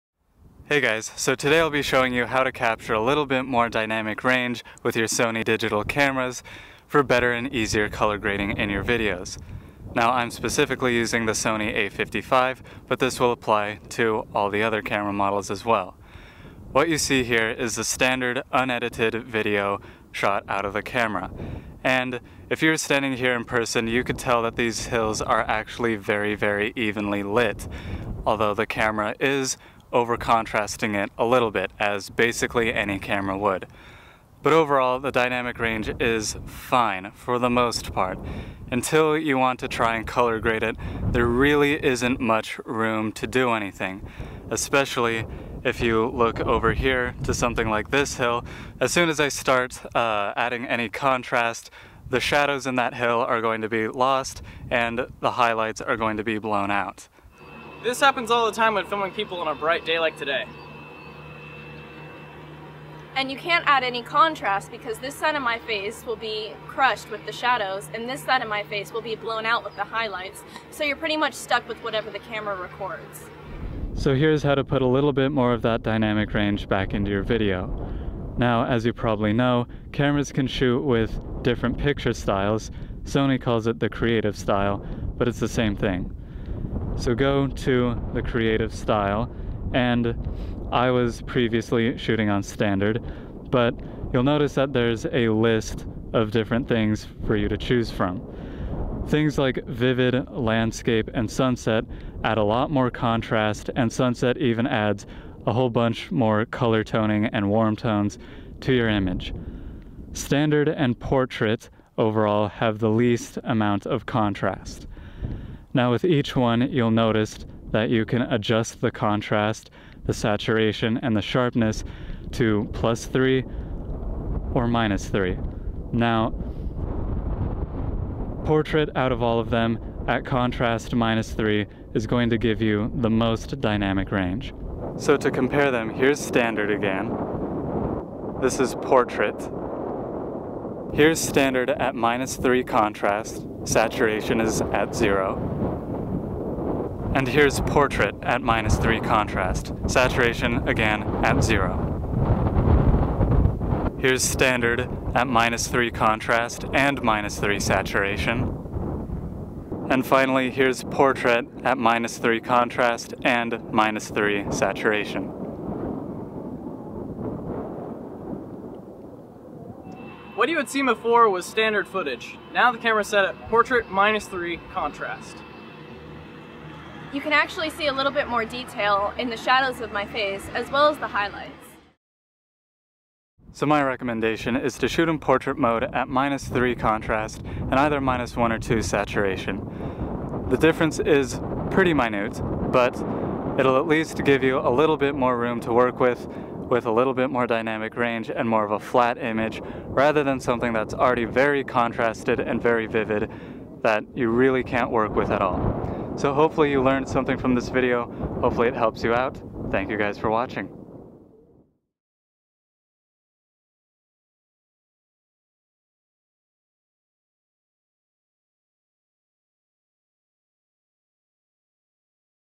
How to Record Better Dynamic Range with Sony DSLRs/DSLTs for Better Color Correction/Grading
A quick tutorial explaining how to capture a more flat image for better and easier color grading in post production.
Filming with the sony cameras, they REALLY contrast your image when you bring it out of the camera, here's how to save a bit more of that dynamic range.

I'm specifically using the a55v, but this applies to all the alpha cameras and other sony digital cameras as well.